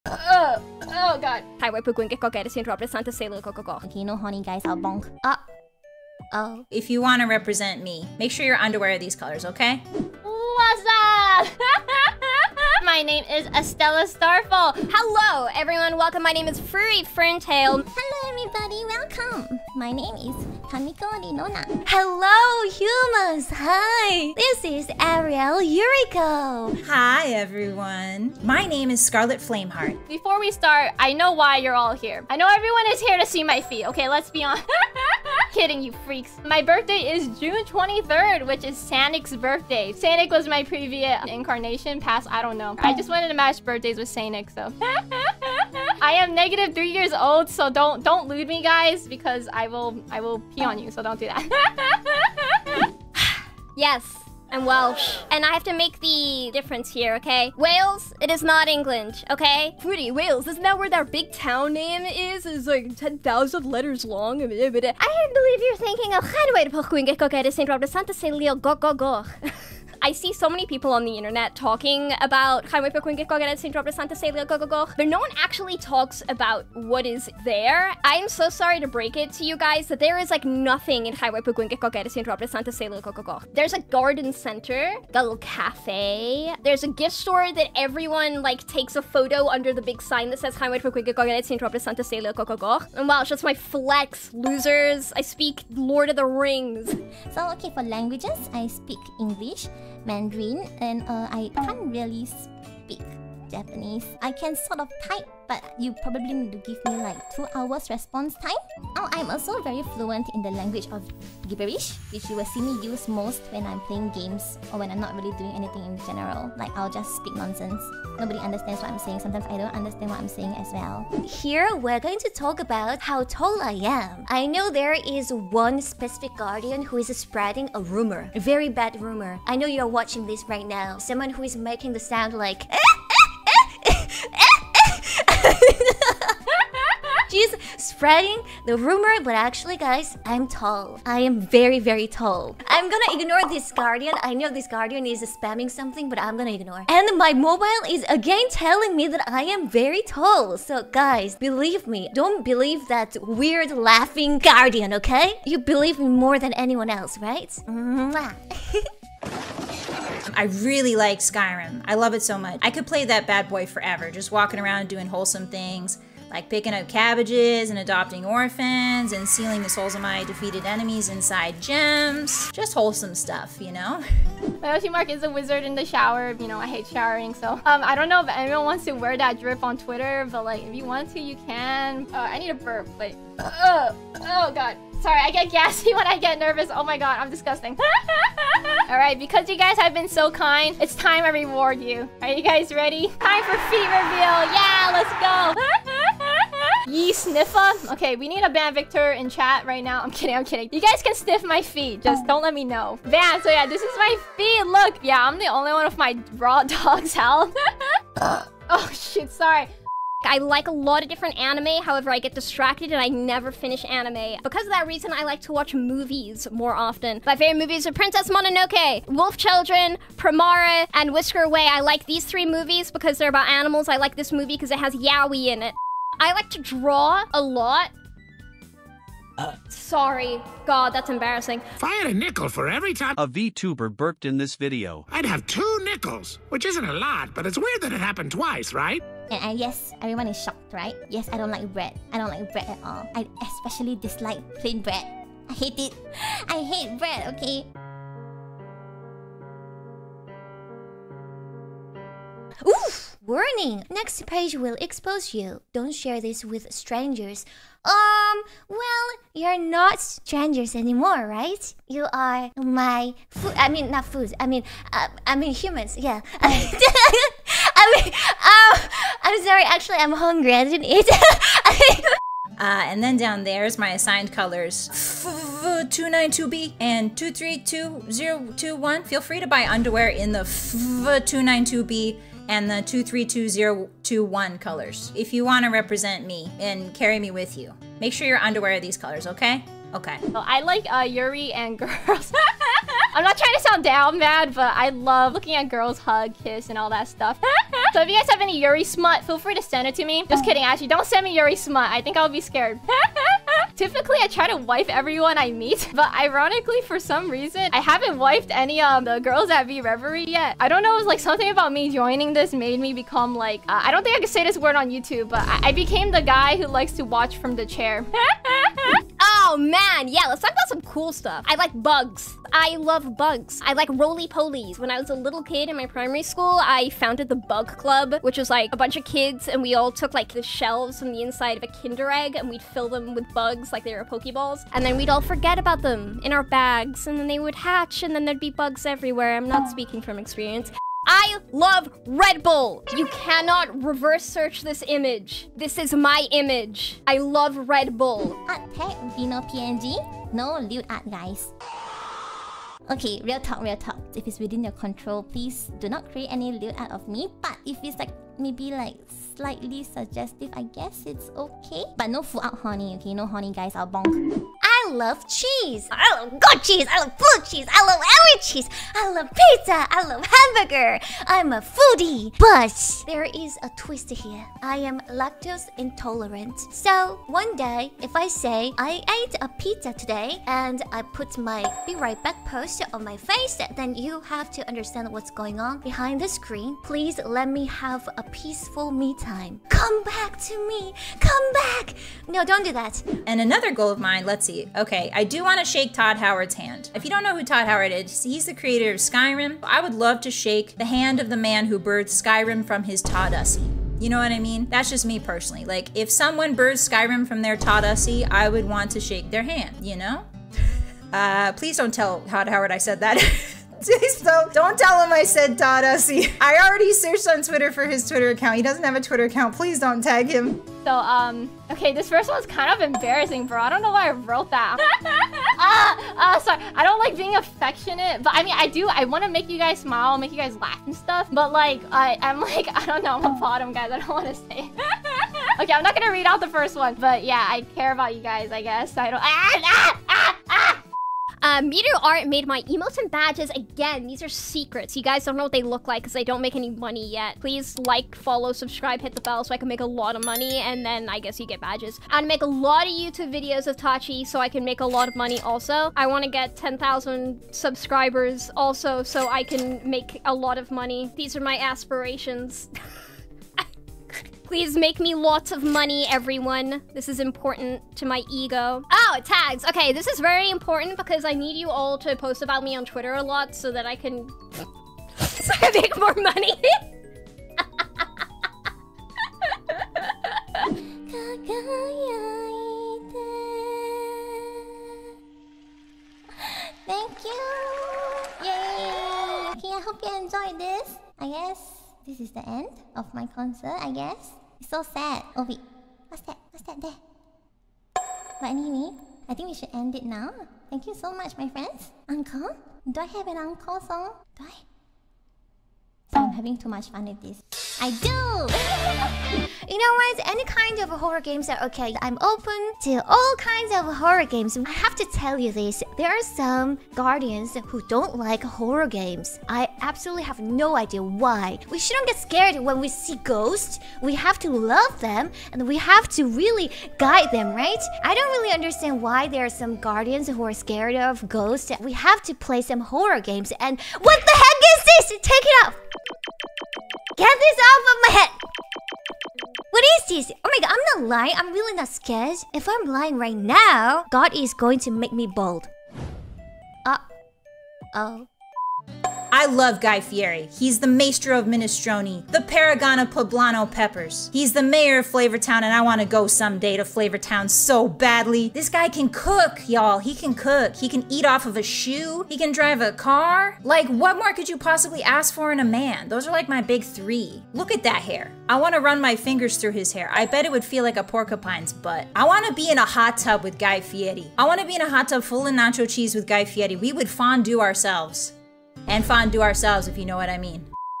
"Make sure your underwear is in this color, okay?" VReverie Guardians Debut Highlights
Please check out the talents!

Estella Starfall: @EstellaStarfall
Furi Ferntail: @FuriFerntail
Kamiko Rinona: @KamikoRinona
Arial Yuriko: @ArialYuriko
Scarlett Flameheart: @ScarlettFlameheart